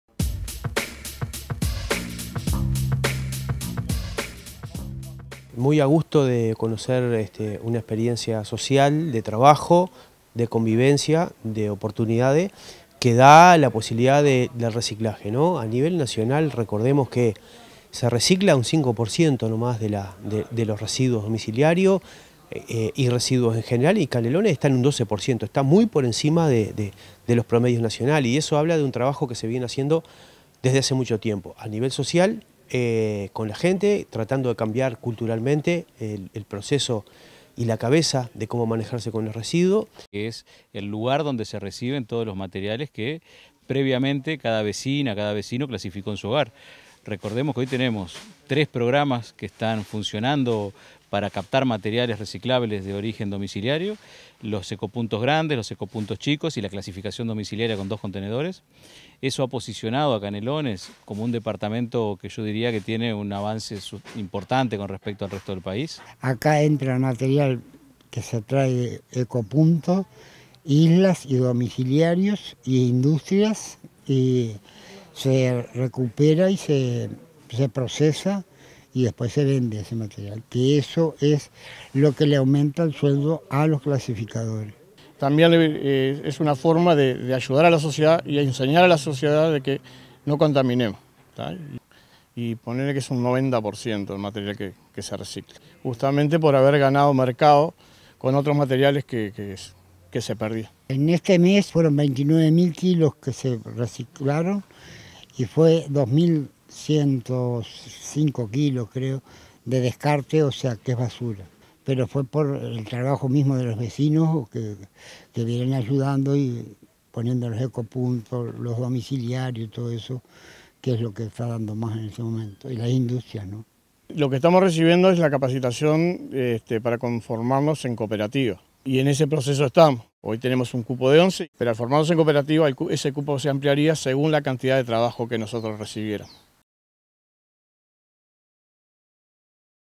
Canelones lidera en reciclaje con el 12,5 % de recuperación de residuos
El Intendente interino de Canelones, Marcelo Metediera, y el Director General de Gestión Ambiental, Leonardo Herou, visitaron el punto de reciclaje ubicado en Paso Escobar esquina Márquez Castro en Solymar norte, Ciudad de la Costa.